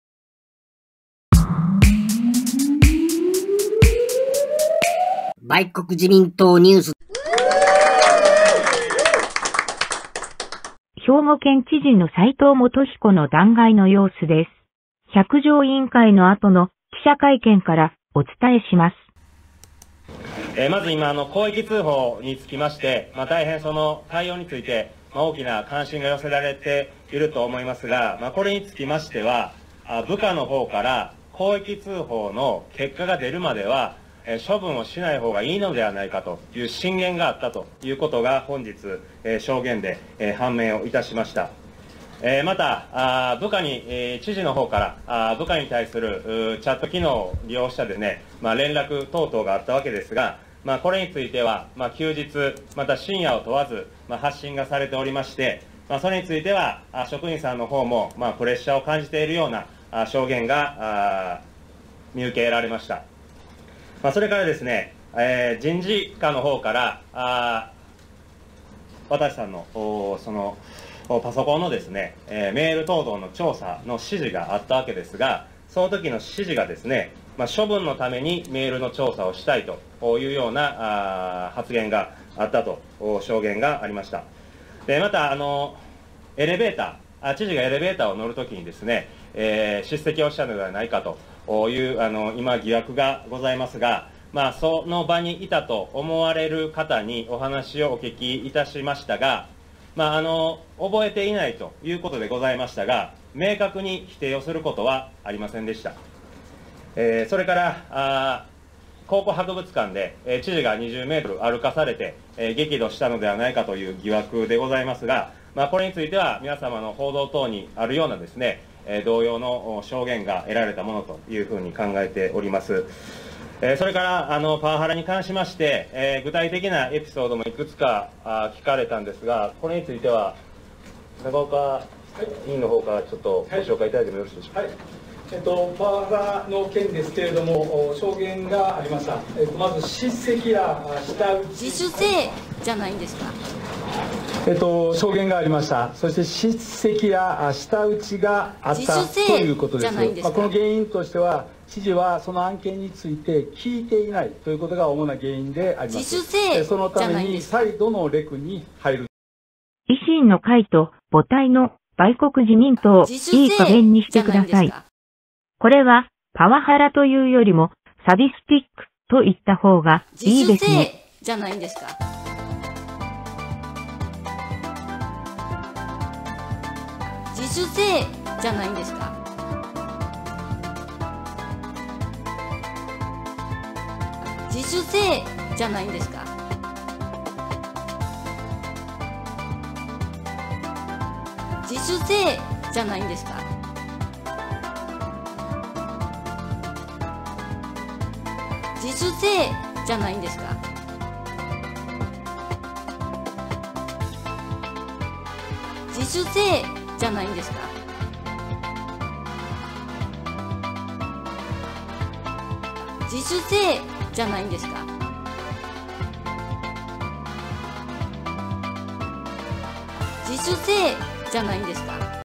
兵庫知事,斎藤元彦の百条委員会,弾劾。パワハラからサディズムに罪名を変更。異常性癖。
引用：兵庫県議会百条委員会。